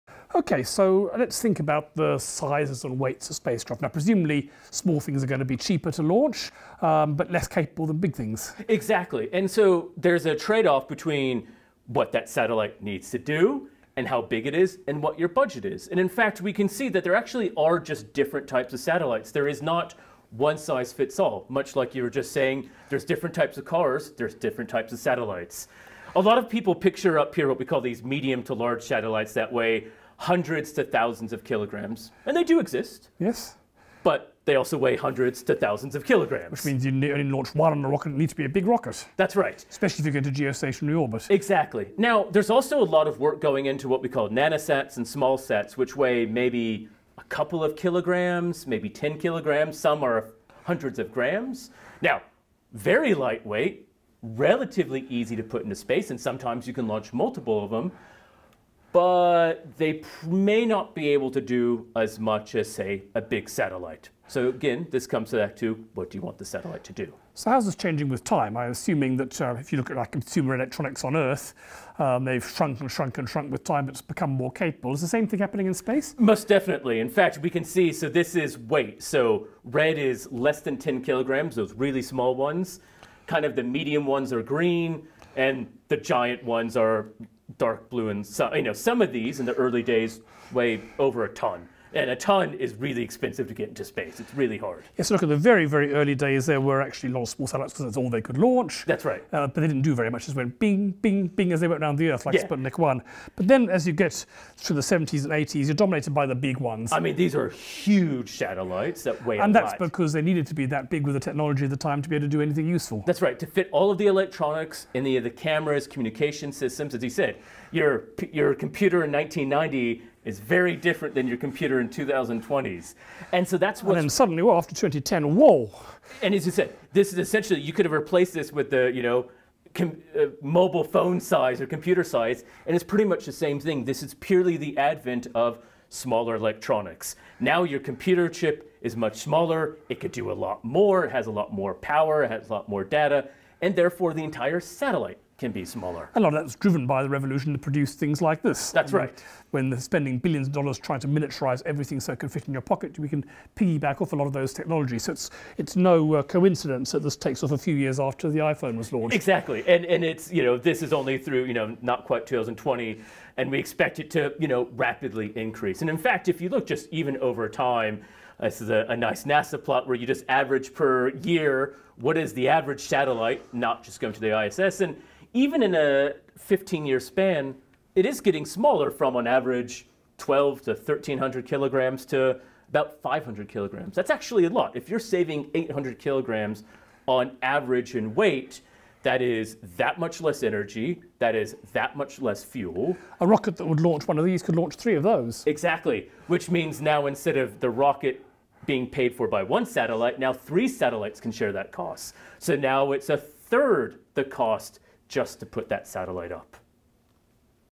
Old Space V2.2 Satellite Sizes
Just how big are satellites and how this is changing over time.